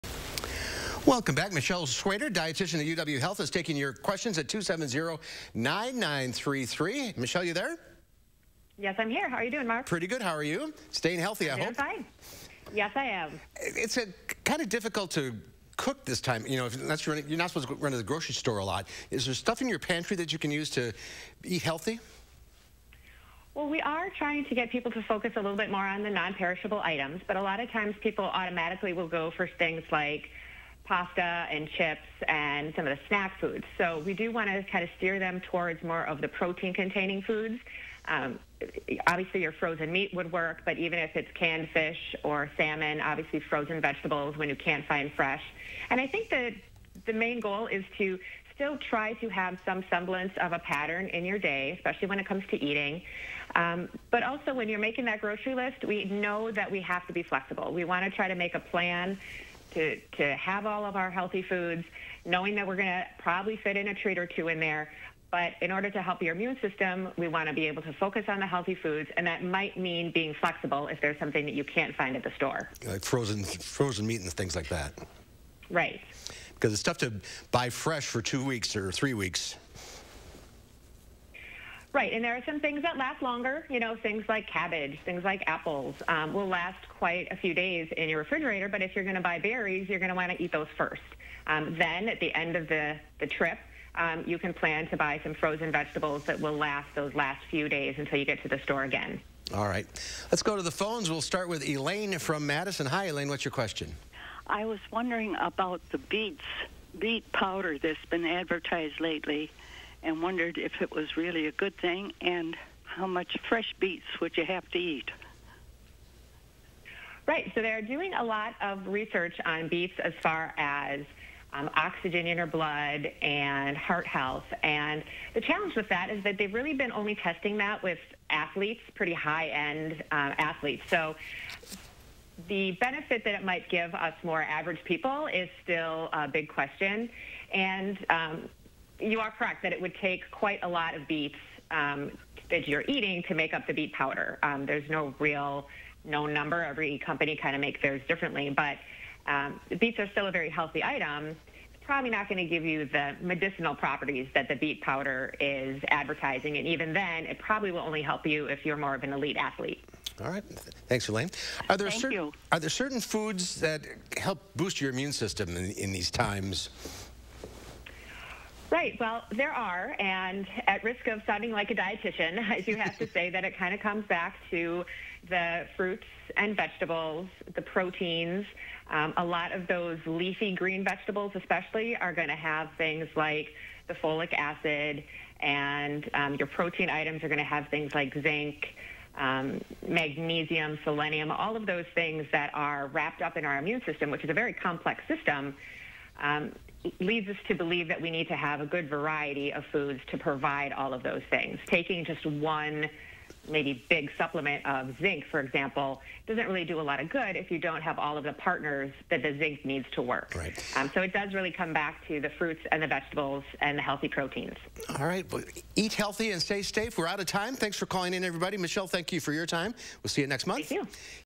Michelle Swader answers your diet questions
Registered dietitian Michelle Swader answers your diet questions.